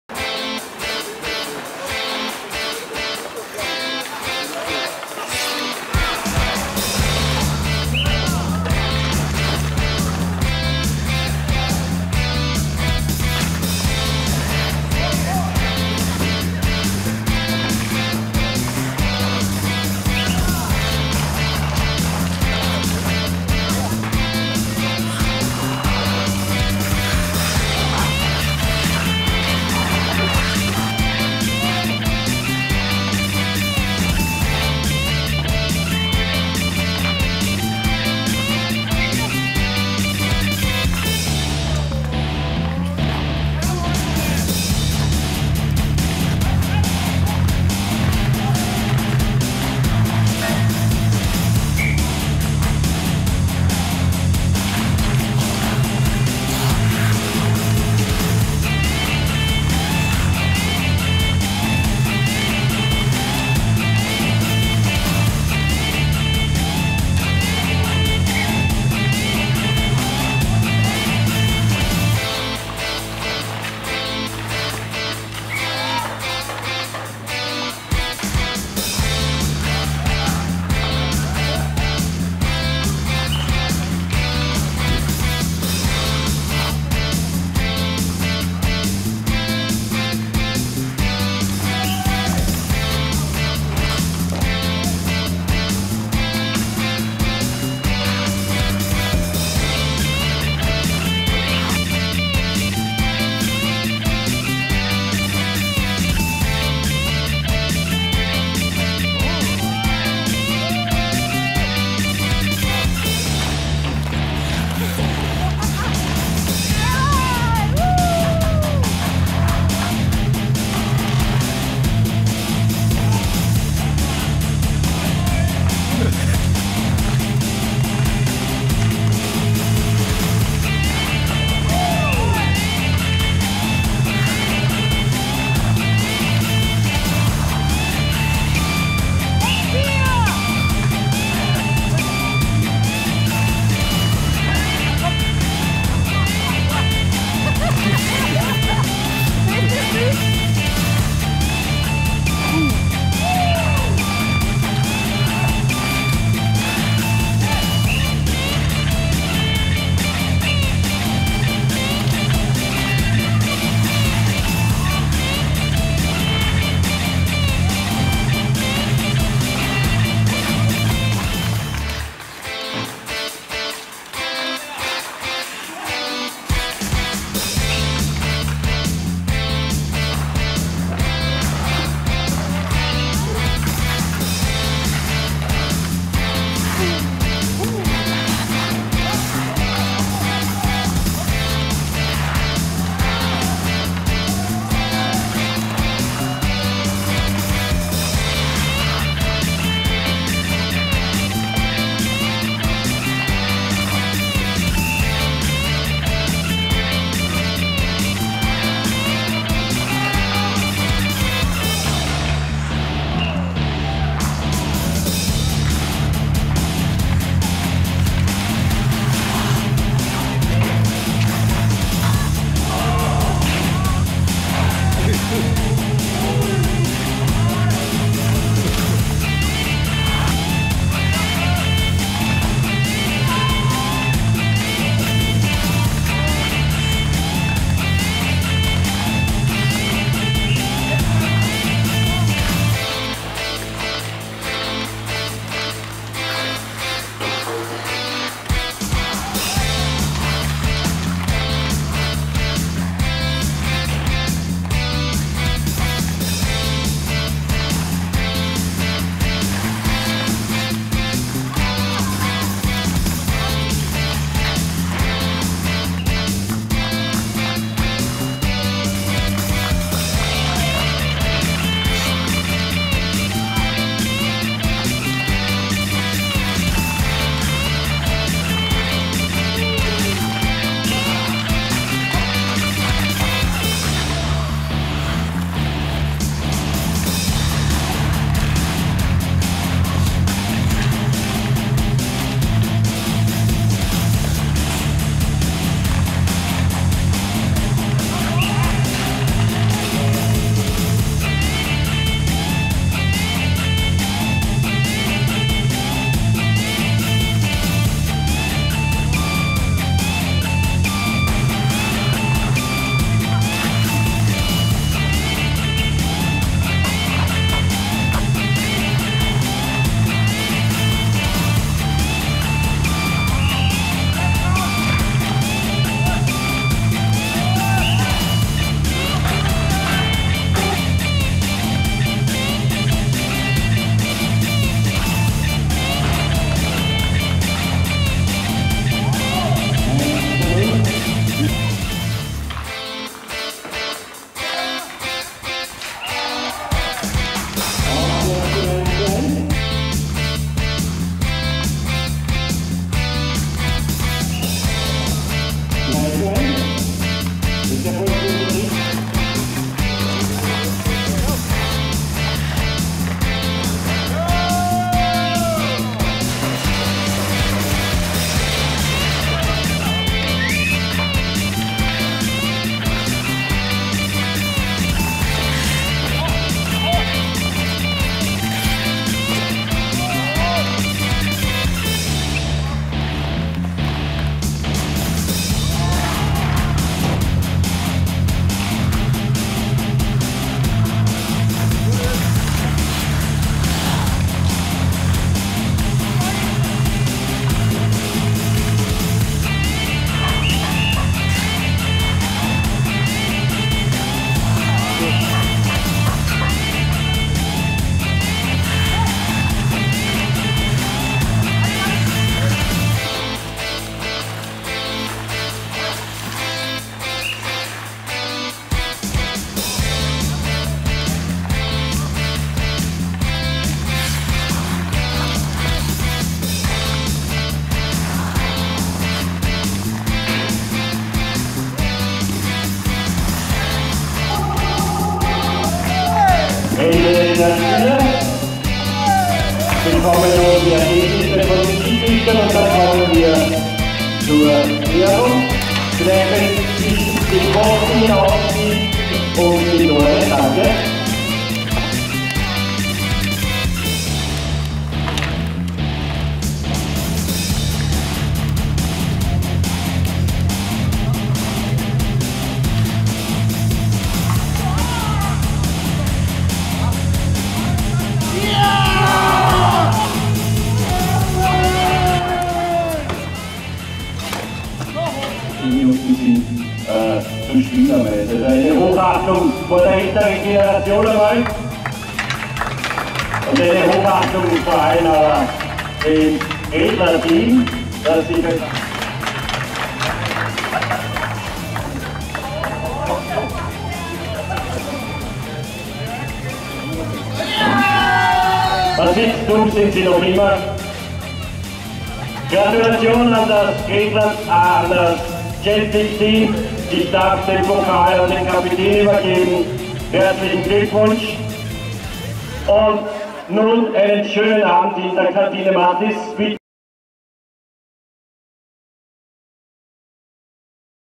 Dragons Cup 2015
Die Highlights vom Dragons Cup 2015 im Rahmen des 27. Internationalen Egon und Maria Mayer Gedenkturniers in der Eisarena Mödling.